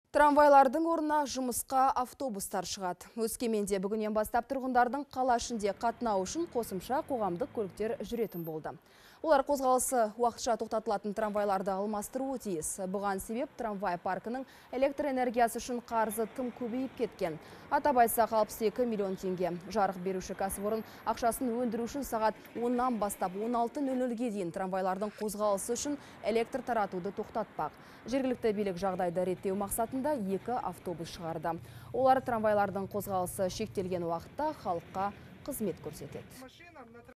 Өскеменде автобустар трамвайларды уақытша алмастырады
Трамвайлардың орнына жұмысқа автобустар шығады.

Өскеменде бүгіннен бастап тұрғындардың қала ішінде қатынауы үшін қосымша қоғамдық көліктер жүретін болды. Олар қозғалысы уақытша тоқтатылатын трамвайларды алмастыруы тиіс. Бұған себеп трамвай паркінің электр энергиясы үшін қарызы тым көбейіп кеткен. Атап айтсақ, 62 миллион теңге. Жарық беруші кәсіпорын ақшасын өндіру үшін сағат 10:00-ден бастап 16:00-ге дейін трамвайлардың қозғалысы үшін электр таратуды тоқтатпақ. Жергілікті билік жағдайды реттеу мақсатында екі автобус шығарды. Олар трамвайлардың қозғалысы шектелген уақытта халыққа қызмет көрсетеді.